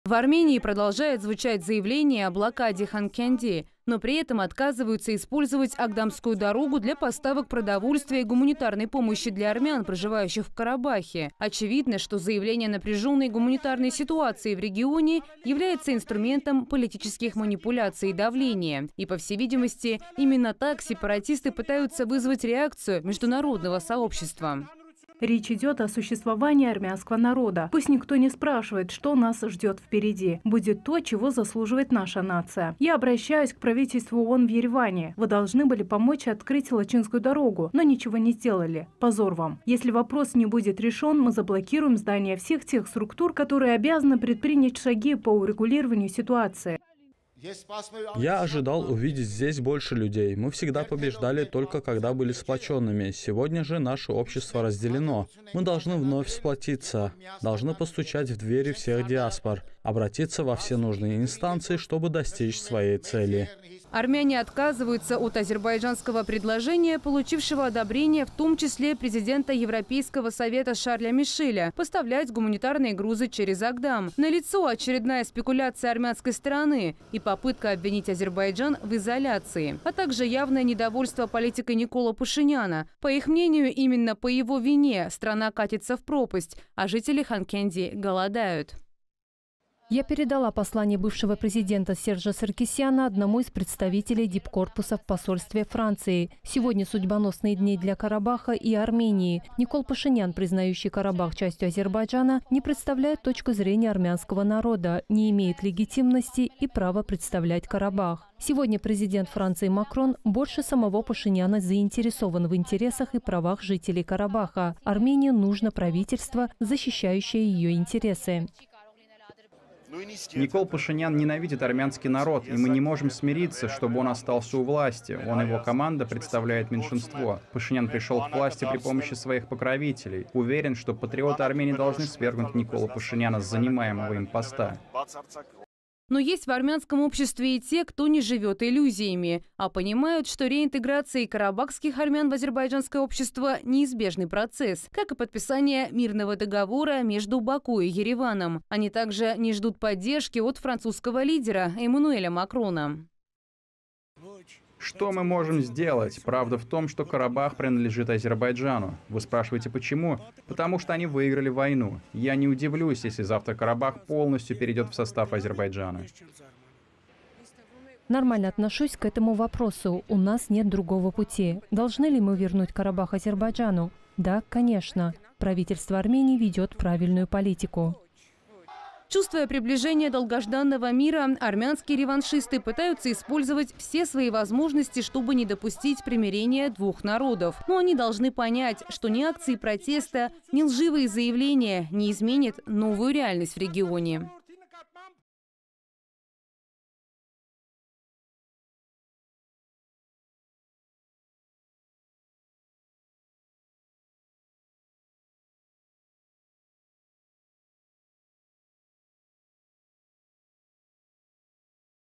Спектакль по-армянски: жители Еревана и Ханкенди вышли на митинги
Чем ближе Азербайджан и Армения к подписанию мирного договора, тем больше претензий высказывают армянские реваншисты. Уже несколько дней в Ереване и Ханкенди проходят акции протеста против реинтеграции армян Карабаха в азербайджанское общество.